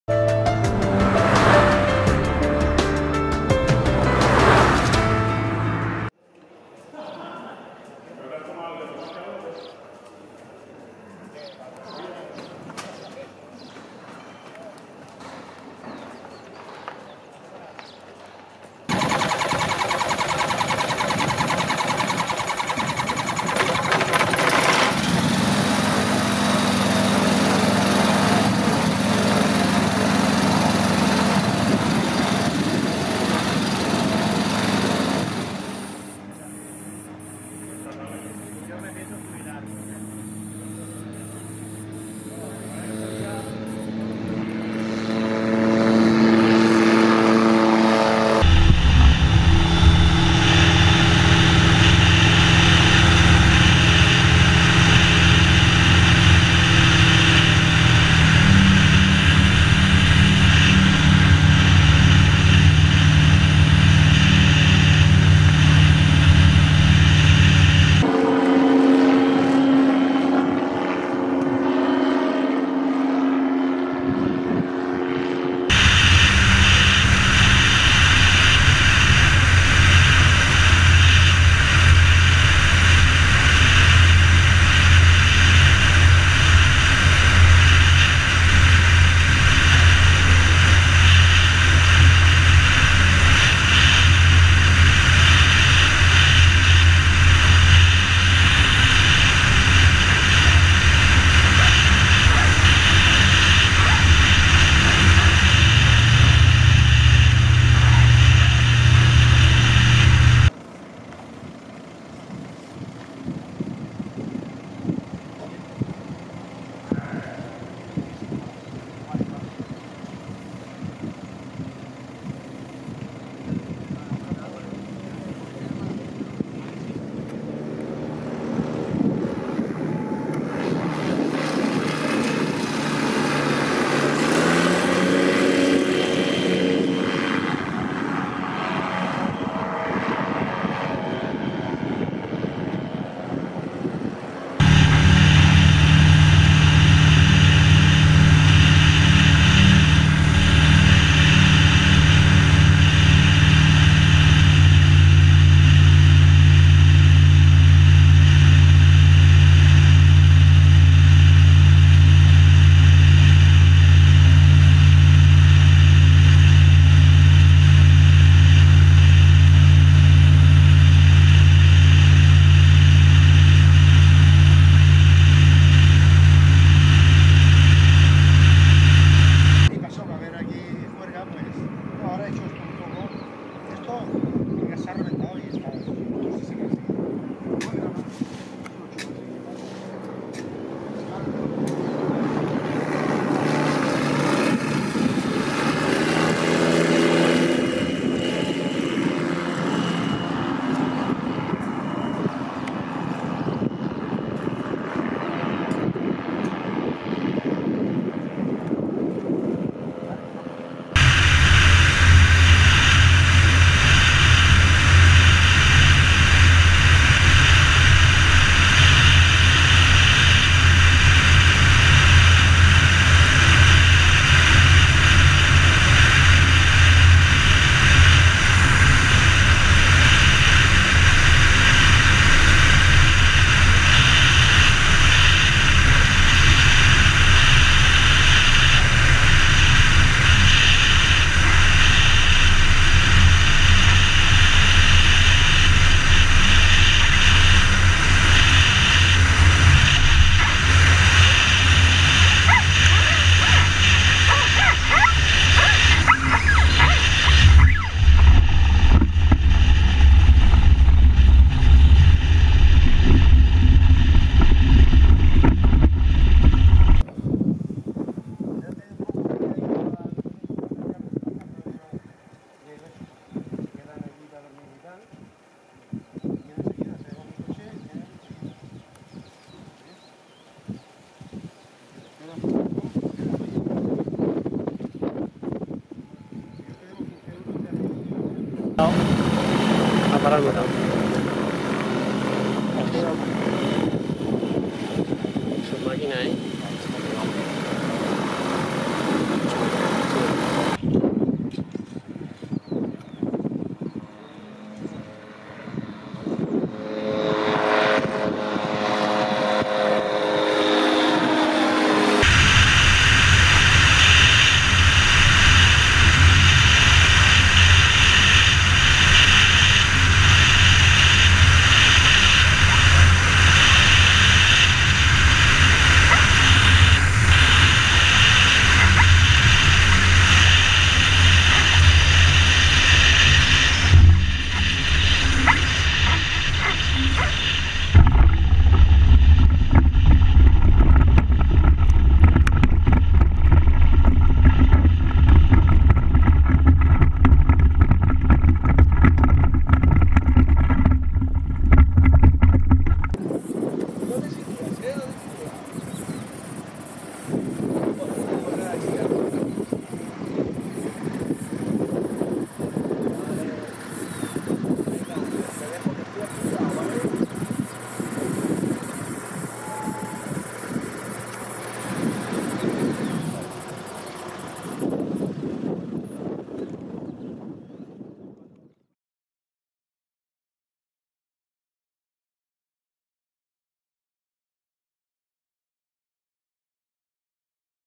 Tanarg 912 Trike Ultralight [4K]
Air Creation Tanarg 912 Trike
Flight over Olocau

Many thanks to Daniel Mellido

Panasonic Lumix DMC FZ1000 4K UHD Video
Gopro 3 + Black Edition 4K UHD Video

Specifications (Tanarg 912 trike with a BioniX 15 wing)

General characteristics

    Crew: one
    Capacity: one passenger
    Wingspan: 9.85 m (32 ft 4 in)
    Height: 3.45 m (11 ft 4 in)
    Wing area: 15.1 m2 (163 sq ft)
    Aspect ratio: 6.4:1
    Empty weight: 249 kg (549 lb)
    Gross weight: 472.5 kg (1,042 lb)
    Fuel capacity: 70 litres (15 imp gal; 18 US gal)
    Powerplant: 1 × Rotax 912ULS four cylinder, liquid and air-cooled, four stroke aircraft engine, 75 kW (101 hp)
    Propellers: 3-bladed Arplast Helice composite, 1.66 m (5 ft 5 in) diameter

Performance

    Maximum speed: 157 km/h (98 mph; 85 kn)
    Cruising speed: 135 km/h (84 mph; 73 kn)
    Stall speed: 55 km/h (34 mph; 30 kn)
    Rate of climb: 6 m/s (1,200 ft/min)
    Wing loading: 30 kg/m2 (6.1 lb/sq ft) maximum[2]